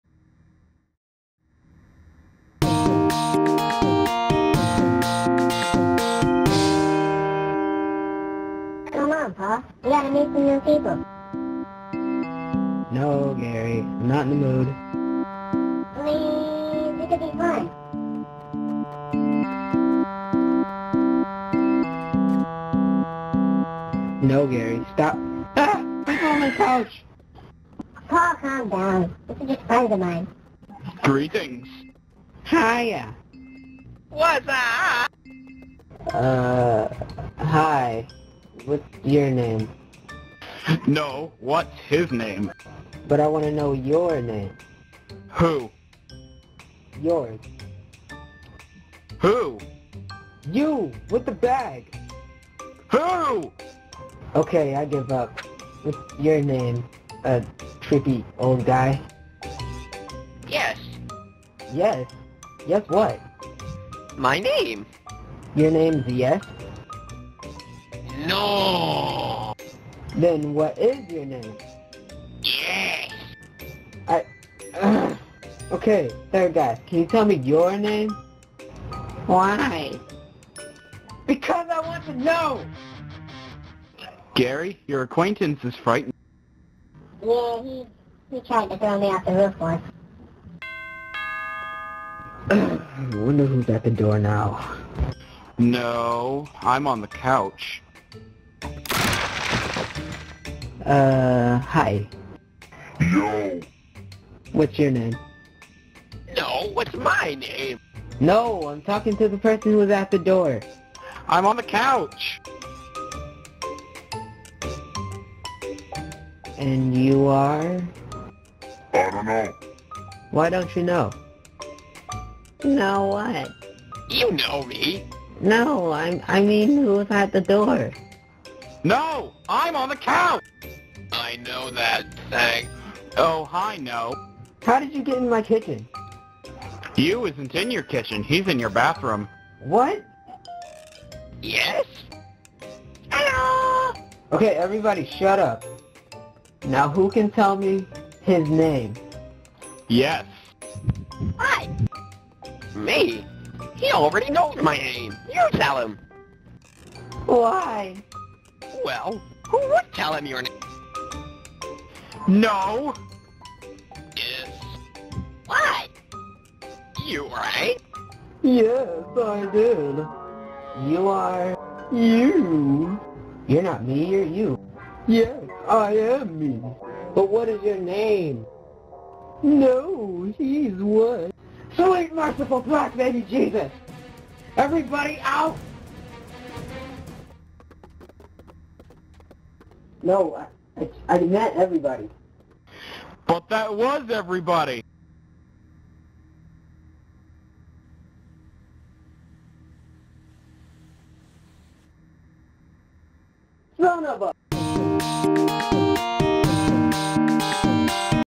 Paul n' Gary Show Short: Who's Who?
Paul tries to comprehend why some random weirdos are suddenly coming into his and Gary's house. Created by De'Lon Warren\Lizzard1102